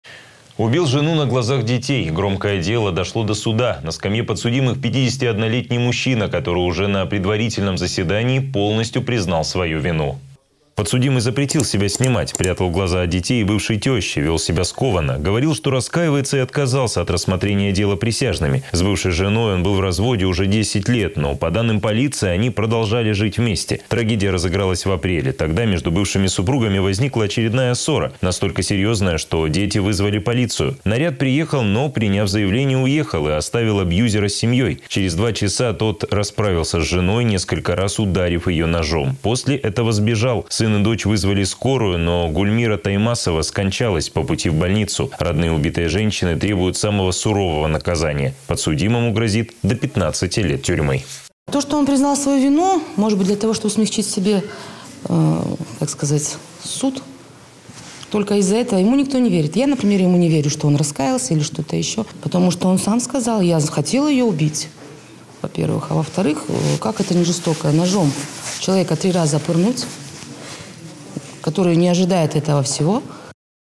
Убил жену на глазах детей
Громкое дело дошло до суда. На скамье подсудимых 51-летний мужчина